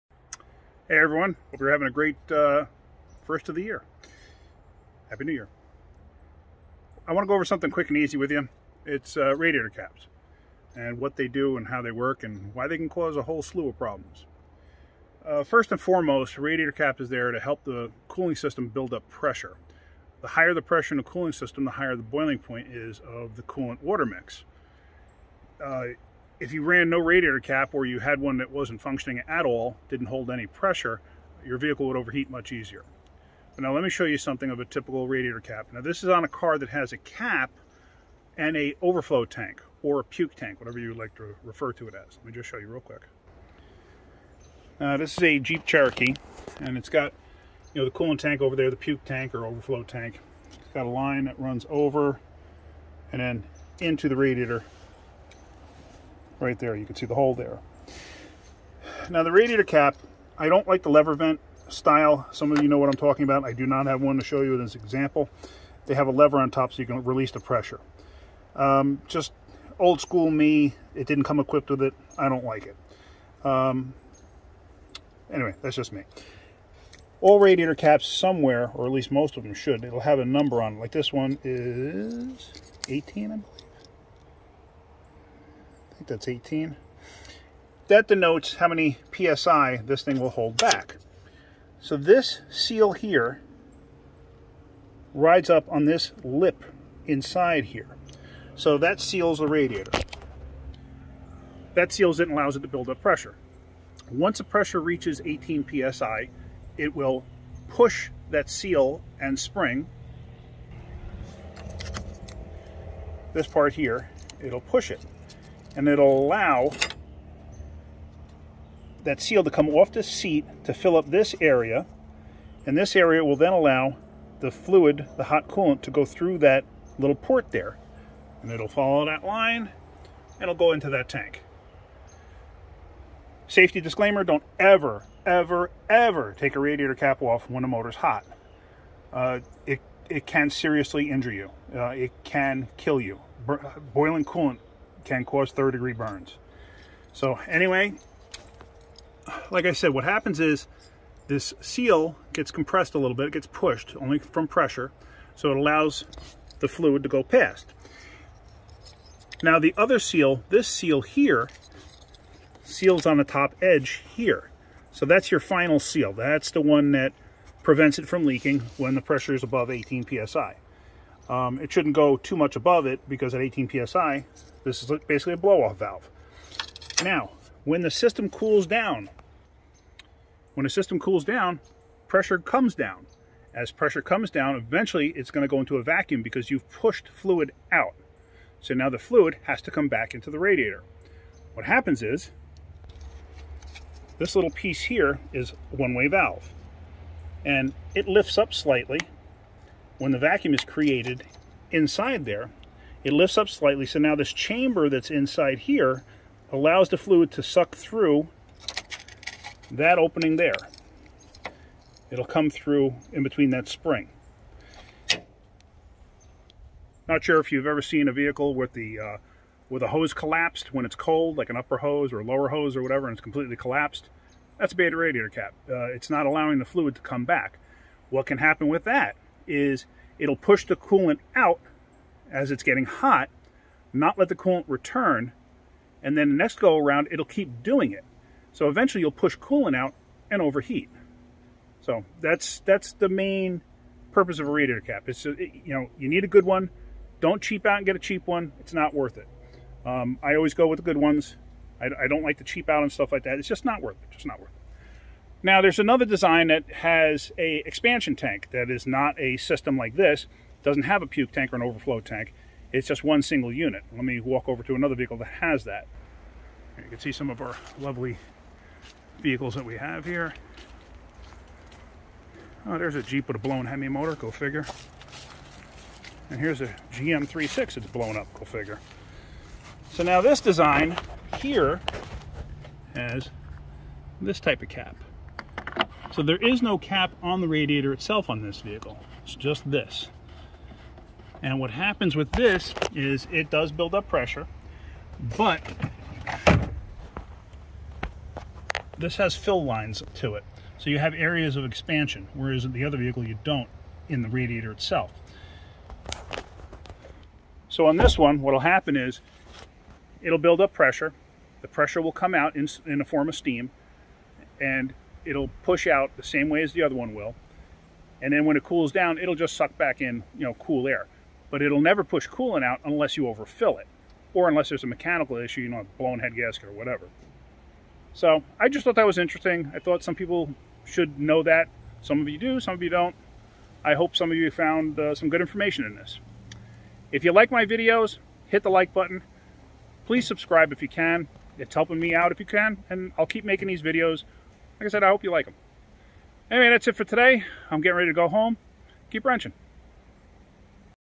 A Discussion about Radiator Caps, their importance and how they work.
I thought this was important information and a good topic for discussion.